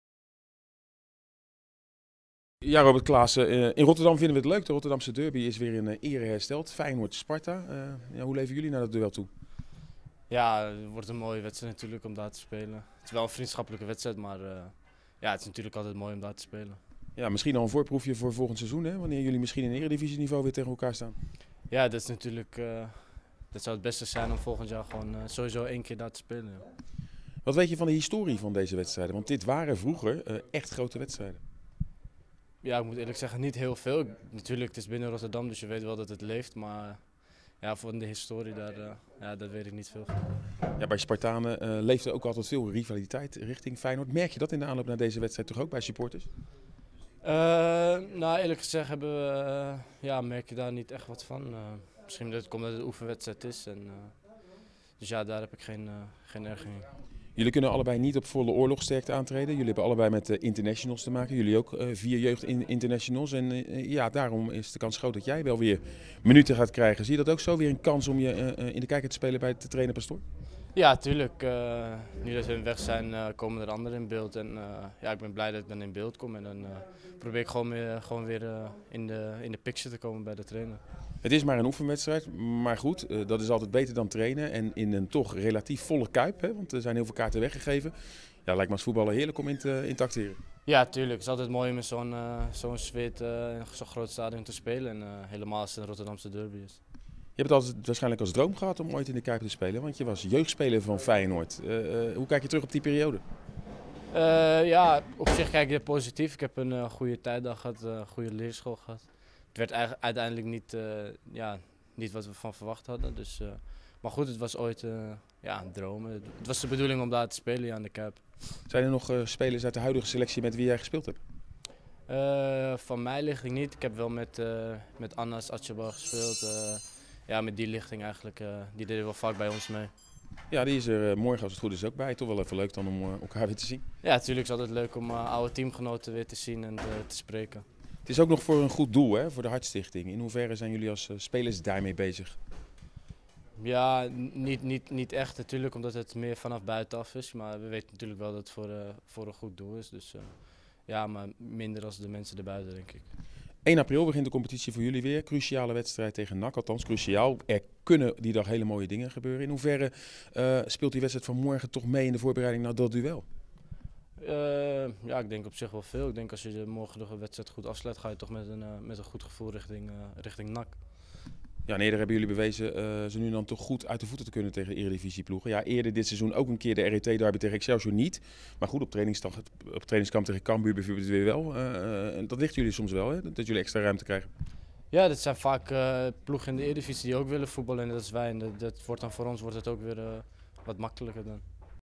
Robert Klaassen over de oefenwedstrijd tussen Feyenoord en Sparta
Sparta neemt het oefenduel met Feyenoord van donderdag volgens trainer Alex Pastoor behoorlijk serieus. Er valt volgens de hoofdcoach dan ook zeker wat te halen in die wedstrijd: "Ik denk dat er voor beide ploegen veel op het spel staat."Onder andere de eer van de stad staat op het spel deze wedstrijd: &quot;Beide ploegen willen de vorm die ze hebben verbeteren ten opzichte van het vorige duel. Daarvoor moet je goed voetballen. Als dat lukt, dan kan er een leuke wedstrijd ontstaan. Het duel leeft bij ons zeker, ik vind het een prachtig affiche. Je speelt deze wedstrijd het liefst in officieel verband.&quot;Sparta kan in de wedstrijd tegen Feyenoord niet beschikken over Denzel Dumfries, Finn Stokkers, Rick van Drongelen en Rick Ketting. Stokkers, Dumfries en Ketting zijn opgenomen in de selectie van Oranje onder 20. Van Drongelen is geselecteerd voor Oranje onder 19.De Rotterdamse stadsderby Feyenoord-Sparta kan je live volgen op Radio Rijnmond. Vanaf 19.00 uur hoor je alles rondom deze oefenwedstrijd, die in het teken staat van de Hartstichting. De aftrap in de Kuip van de &apos;Benefietwedstrijd voor het leven&apos; is om 20.00 uur.